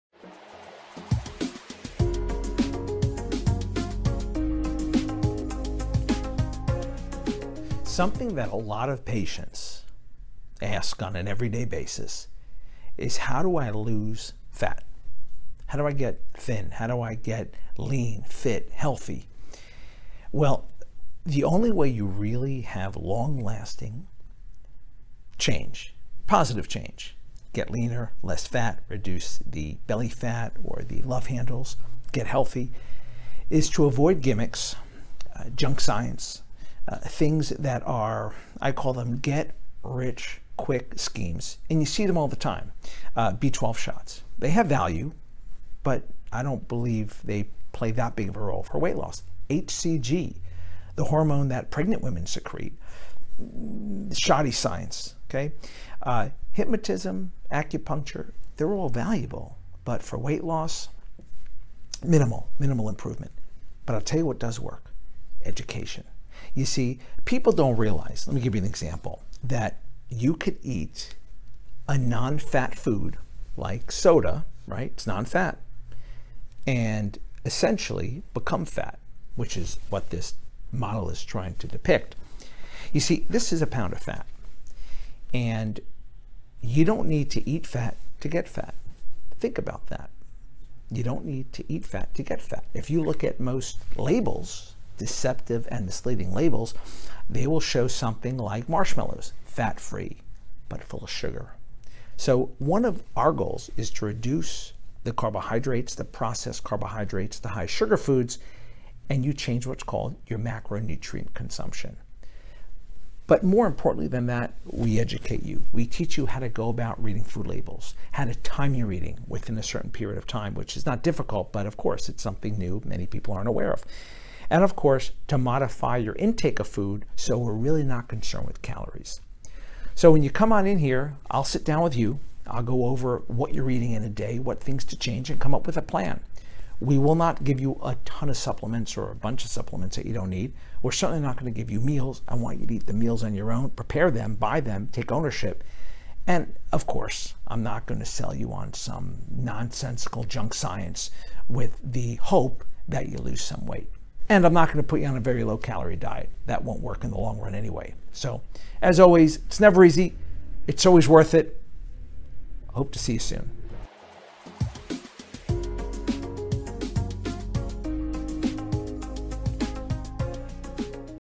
How We Help Patients Lose Body Fat Sustainably
An overview how our weight loss program works.